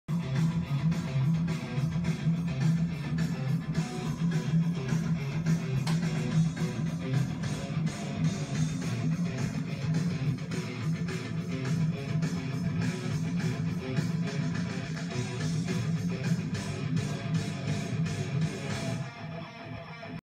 Burmese cat reacts to Metallica
Louis the Burmese cat, looking pretty annoyed at being woken up by Metallica